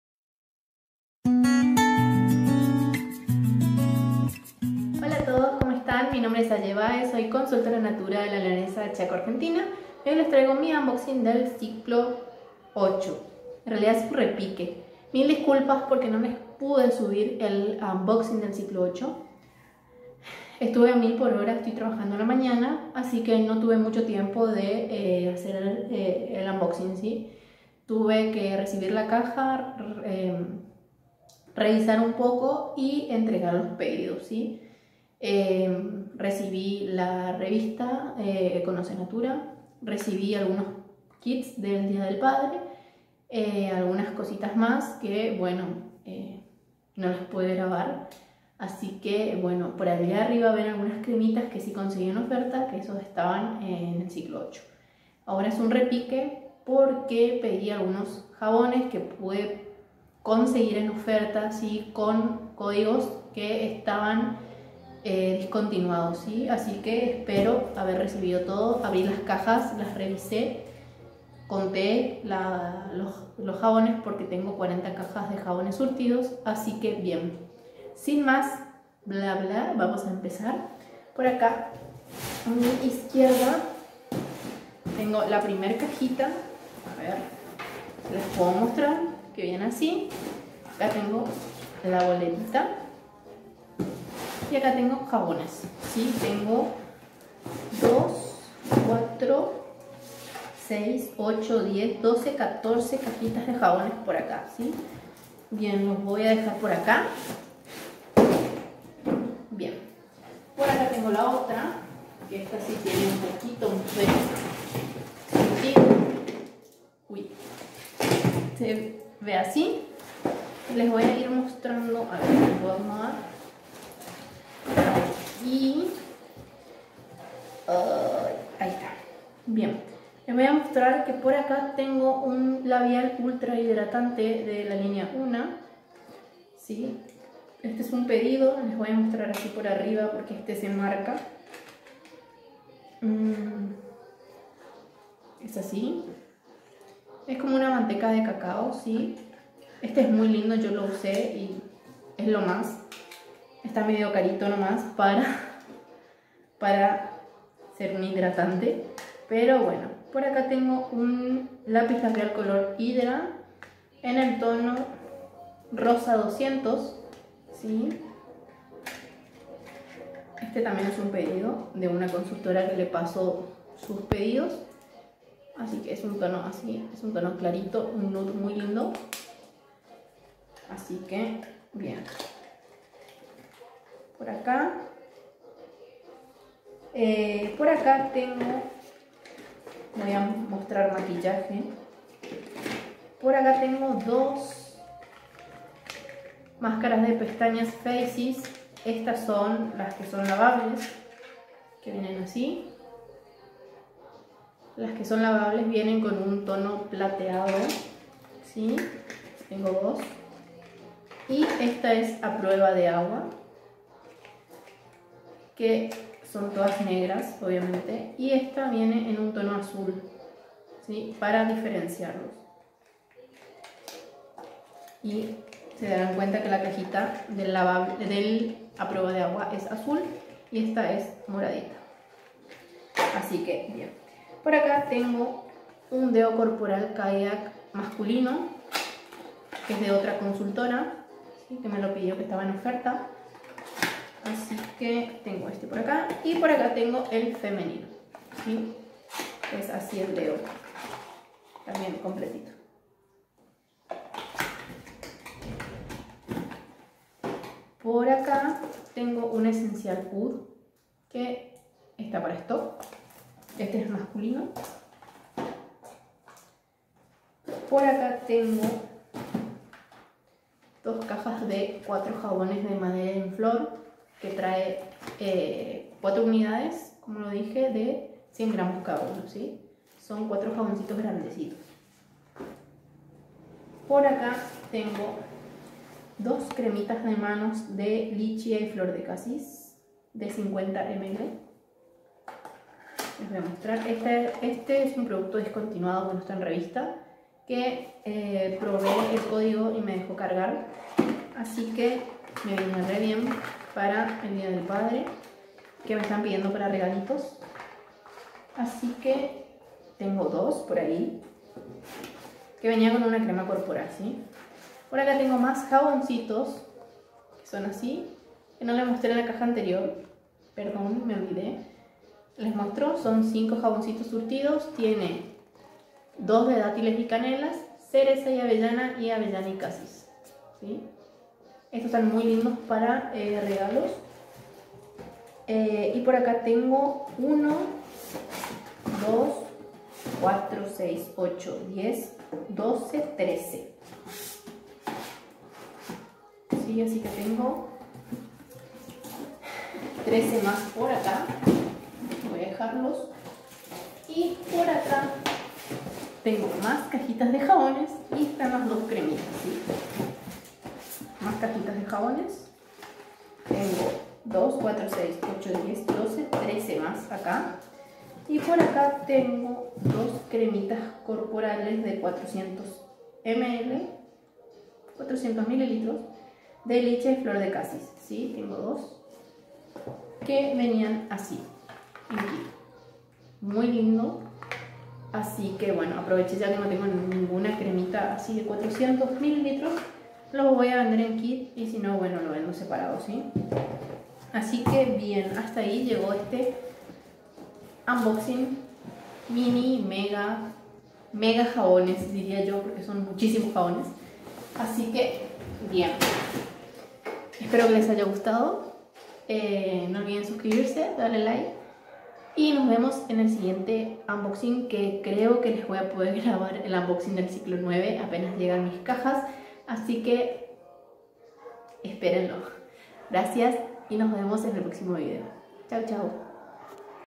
Repique Unboxing ciclo 8 2021 Argentina🤯 Muchísimos jabones😍😍
Hola a todos como estan? Les dejo mi video, espero les guste, cualquier consulta pueden dejarme en los comentarios